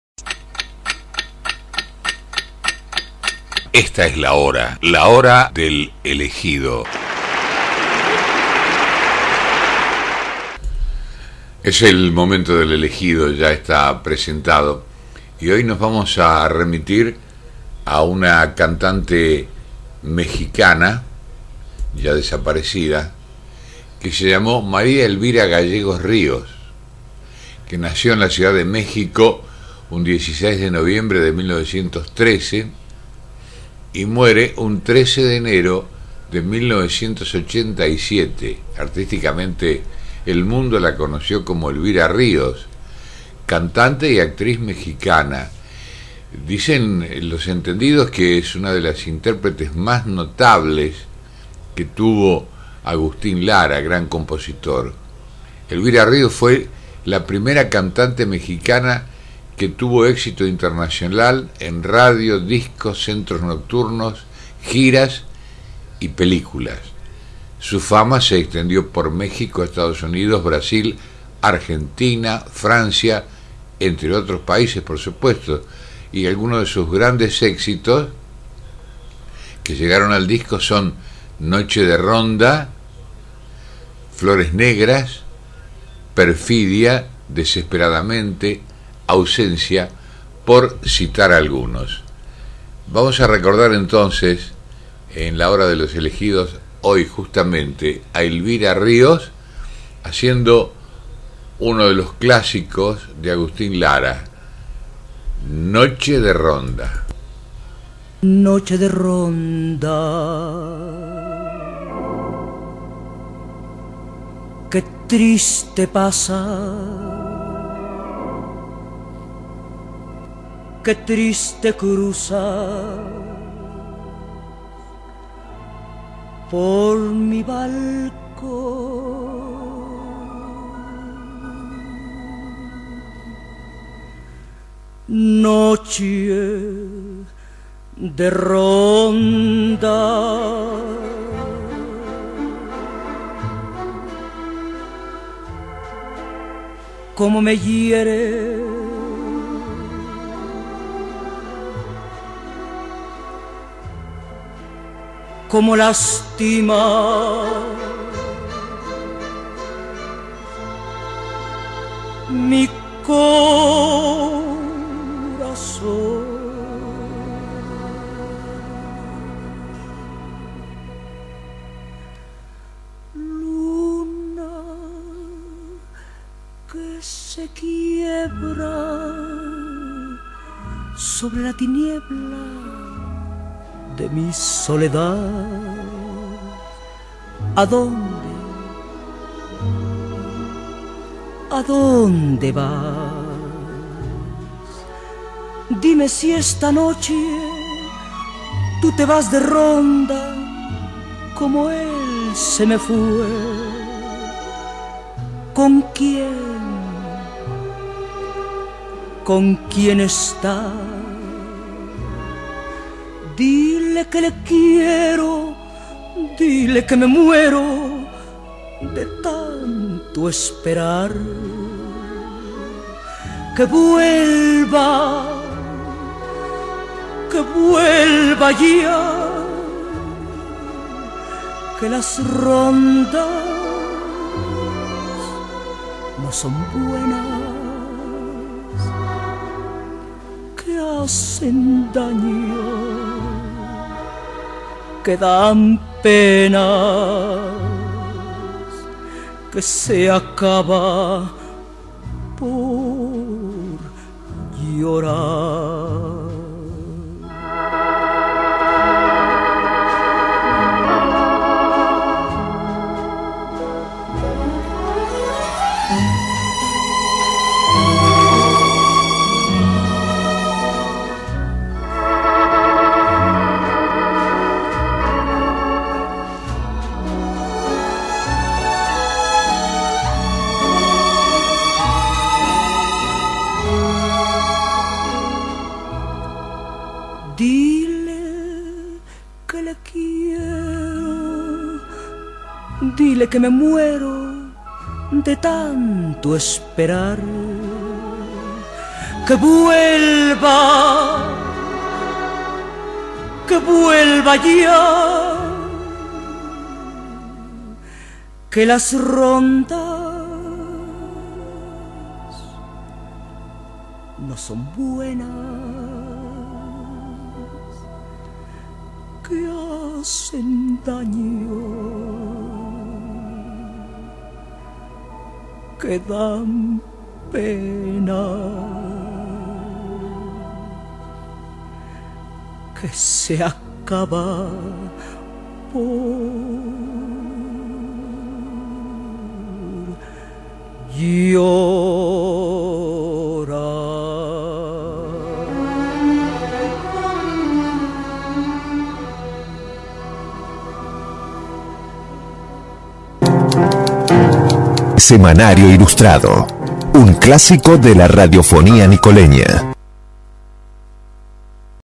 Elegido del 13 de Enero 2018 Elvira Ríos
Semanario Ilustrado recordó a Elvira Rios. en su edición del  13 de Enero de 2018.